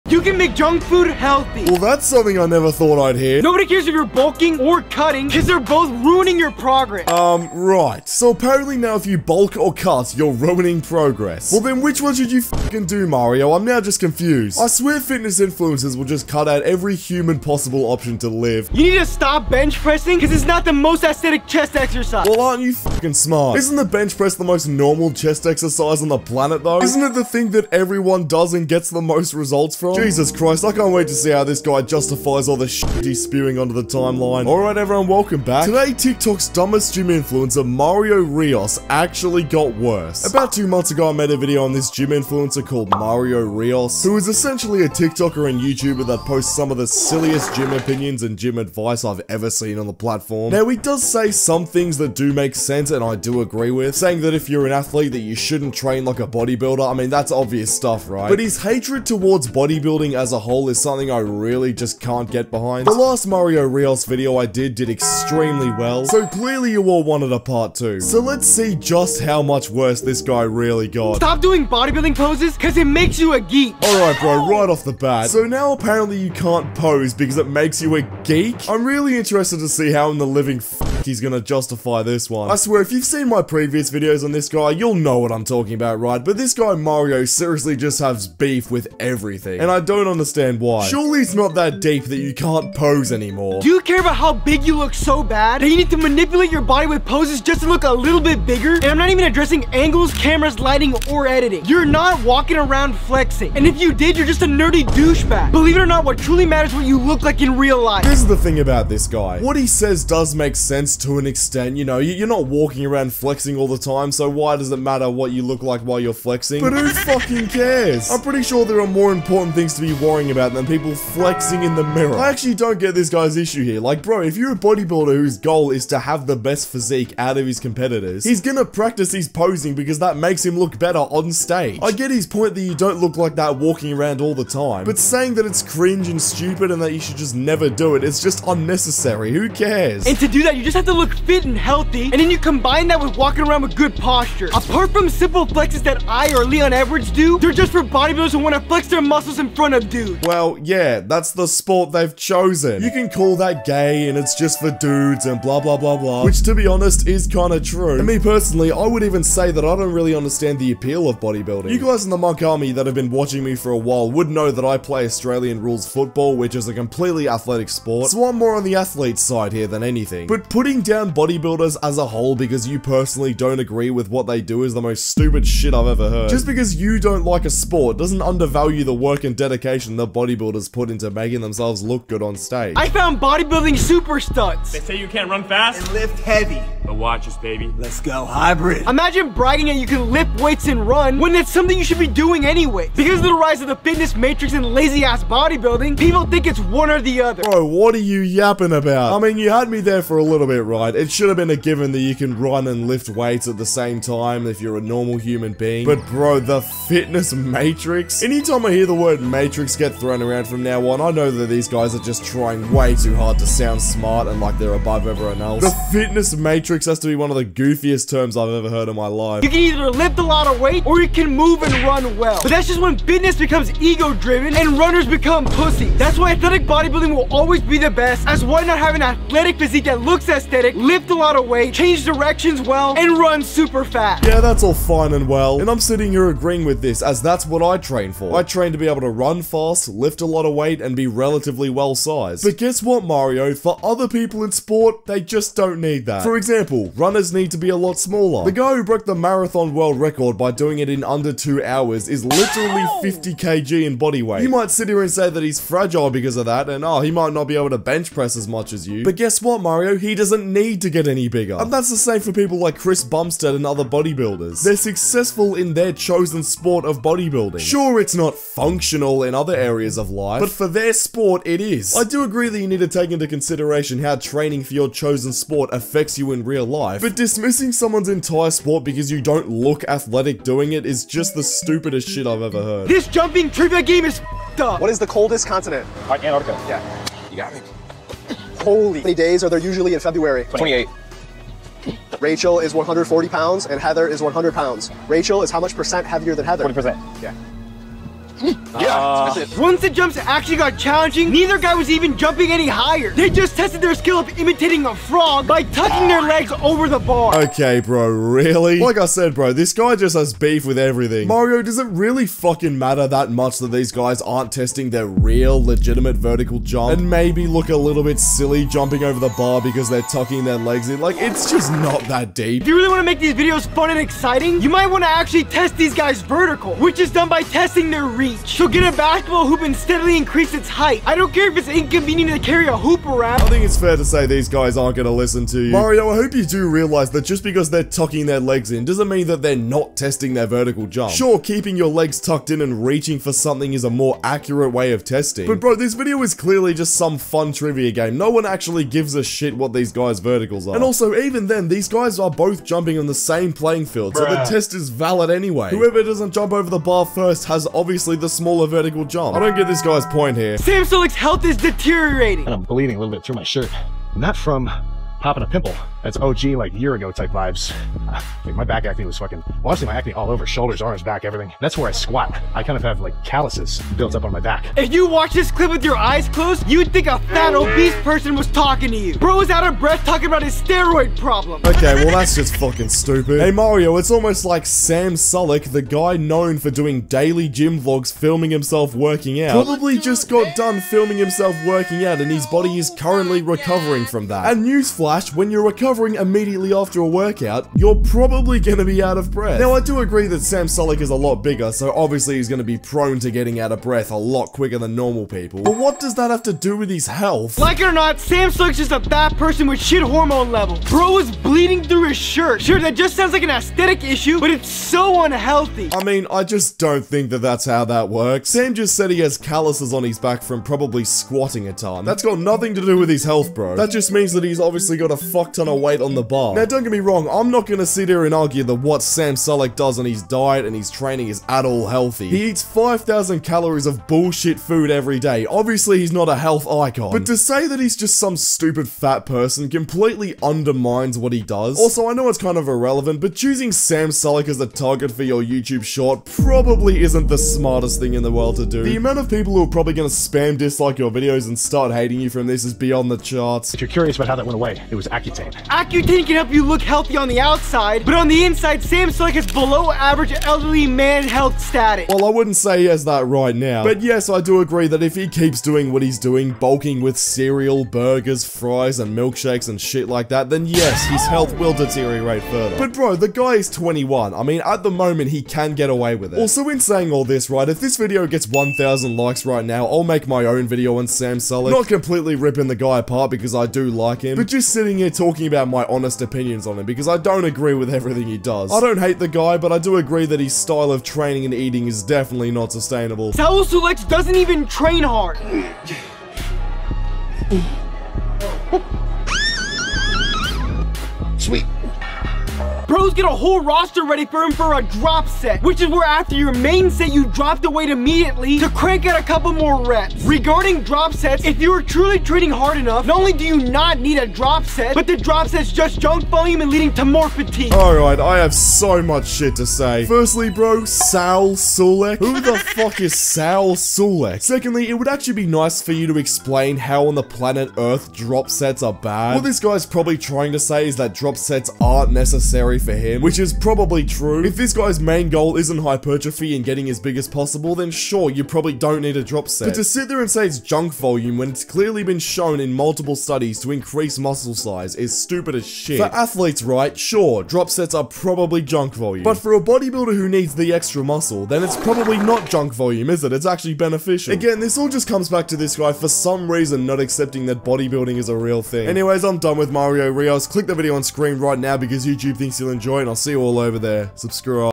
TikTok’s Dumbest Gym Influencer Got Worse!
TikTok’s Dumbest Gym Influencer Got Worse! @Coach_MarioRios

This is tiktok's dumbest gym influencer mario rios, brother of martin rios and tiktokers silliest gym influencer, this guy mario posts some of the worst gym advice and opinion videos and he somehow got worse!